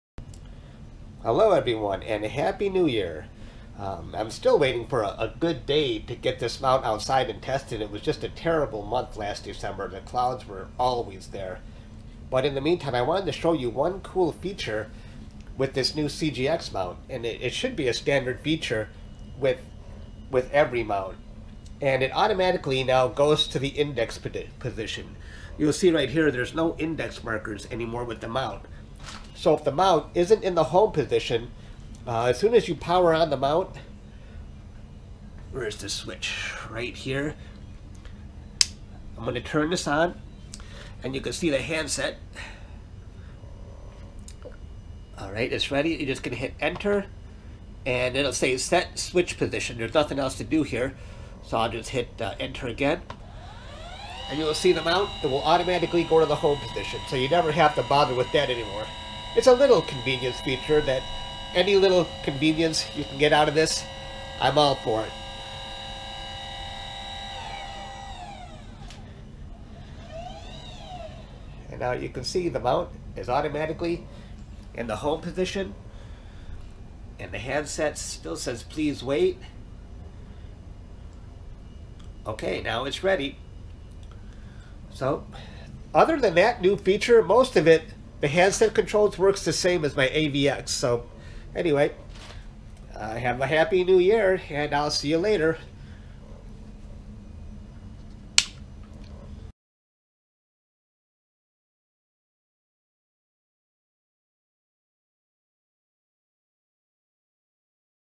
Celestron CGX Mount Automatic Index Position
The Celestron CGX mount now has home sensors that allow the mount to always start in the index position regardless of orientation before a power reset.  The handset will say "Set Switch Pos" - from there just hit "Enter".  It's a small convenience feature that I will probably get used to real fast.  Happy New Year!